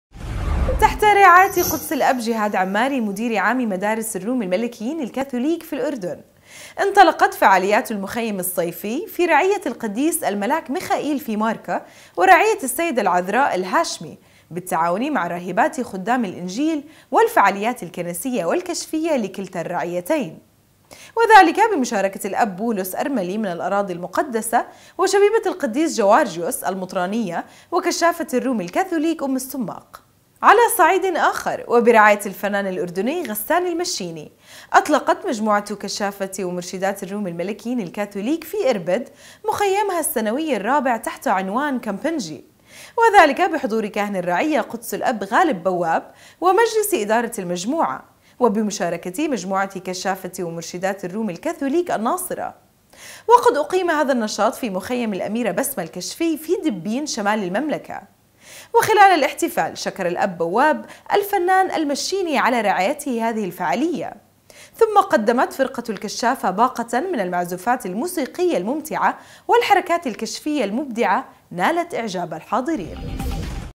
نورسات الأردن : أطلقتْ مجموعةُ كشافةِ ومرشداتِ الروم الكاثوليك في إربد مخيمَها السنويَ الرابع
على صعيد آخر وبرعاية الفنان الاردني غسان المشيني أطلقتْ مجموعةُ كشافةِ ومرشداتِ الروم الملكيينَ الكاثوليك في إربد مخيمَها السنويَ الرابع تحت عنوان - كامبنجي - وذلك بحضور كاهنِ الرعية قدسِ الأب غالب بواب ومجلسِ إدارةِ المجموعة وبمشاركةِ مجموعةِ كشافةِ ومرشداتِ الرومِ الكاثوليك - الناصرة ، وقد أقيمَ هذا النشاط في مخيم الأميرة بسمة الكشفي في دبين شمالِ المملكة ، وخلالَ الاحتفال شكر الأب بواب الفنانَ المشيني على رعايتهِ هذه الفعاليه ، ثم قدمت فرقةُ الكشافة باقةً من المعزوفاتِ الموسيقيةِ الممتعة والحركاتِ الكشفيةِ المبدعه نالتْ اعجابَ الحاضرين .